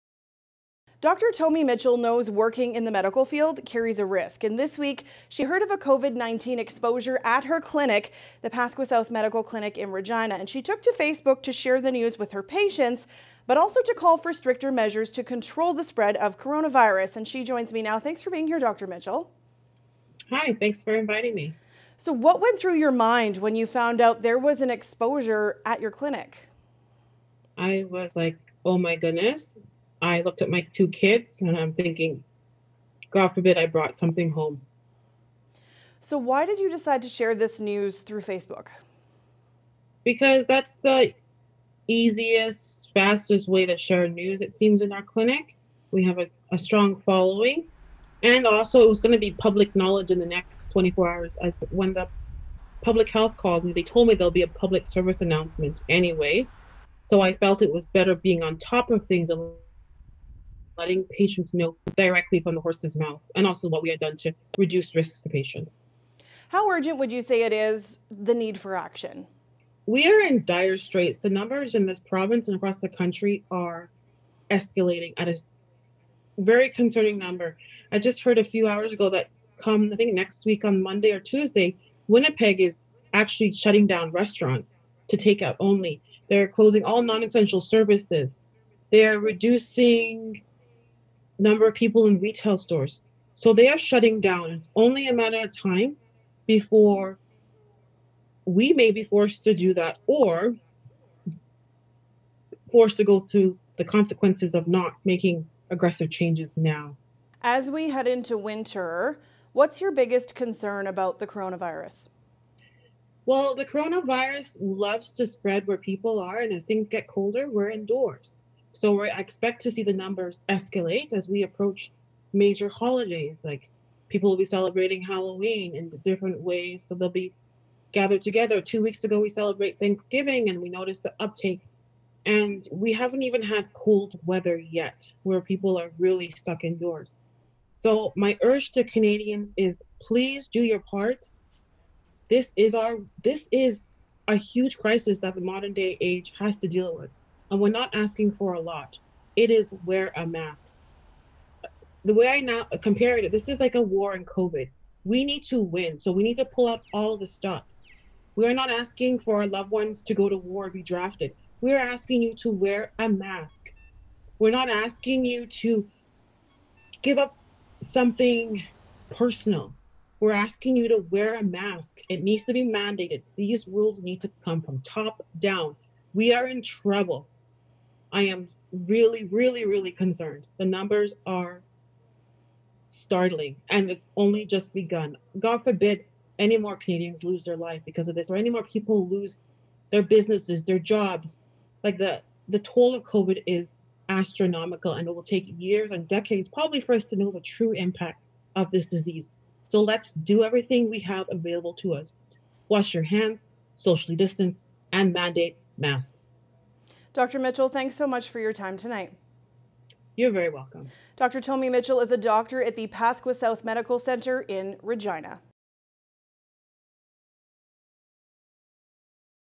A Regina Doctor wants tighter COVID-19 restrictions after an exposure at her clinic
Dr Tomi Mitchell worries about health care system being overwhelmed.